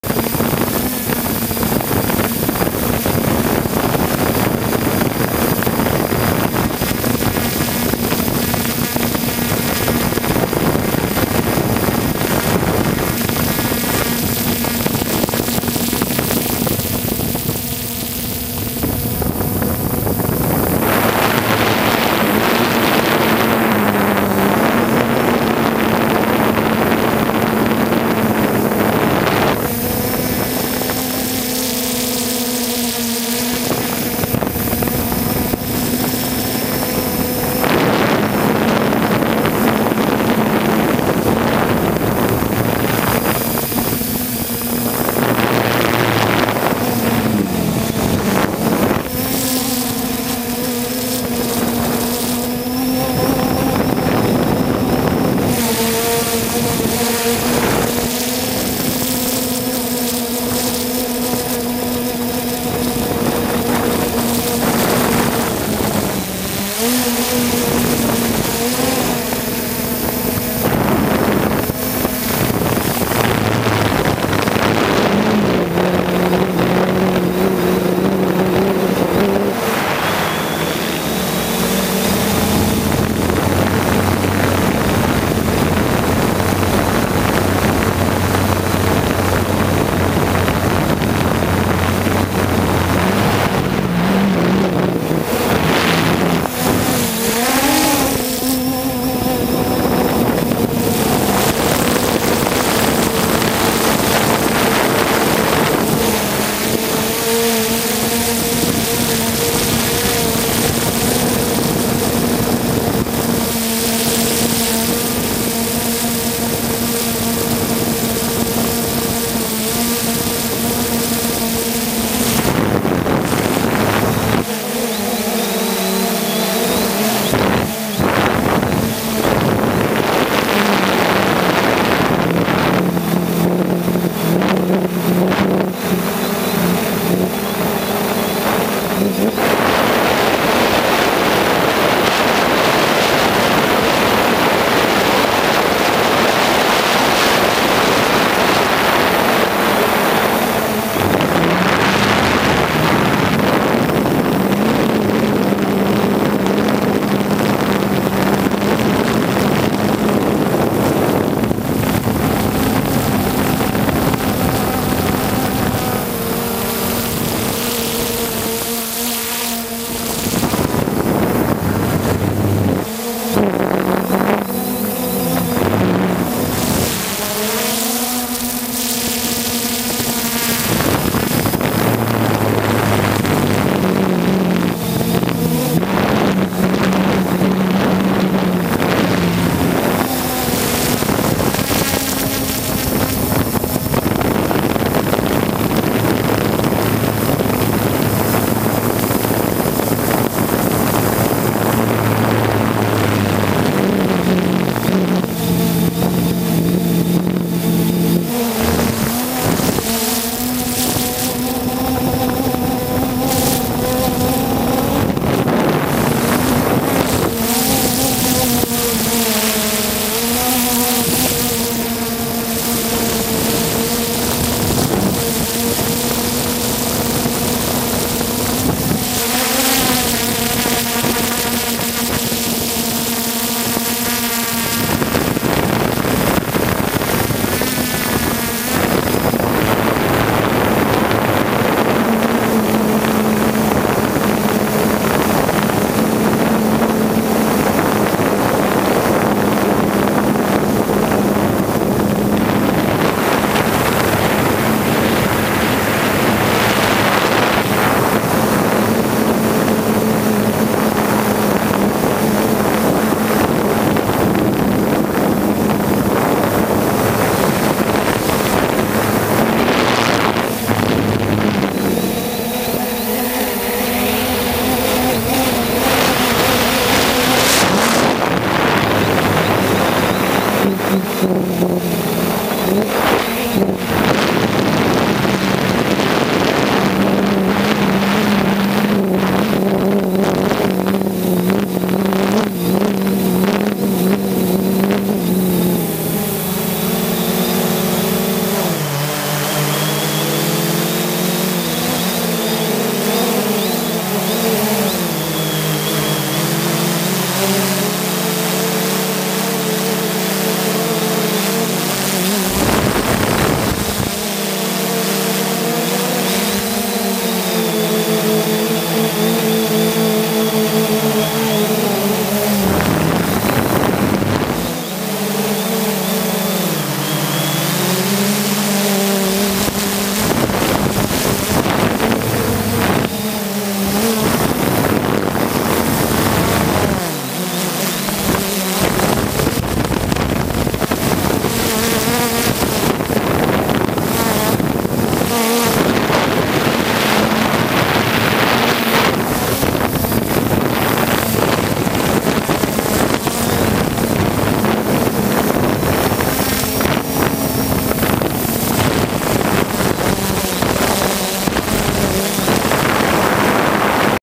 Dawn flying with the 3dr Solo and the Moon RAW
Dawn flight with the drone.
4:25 mark Sun!
Still no Live video, (can't see, so just testing/pointing.
Cleaned the dirty lens as well.
1080.30fps.HD.Gopro3.White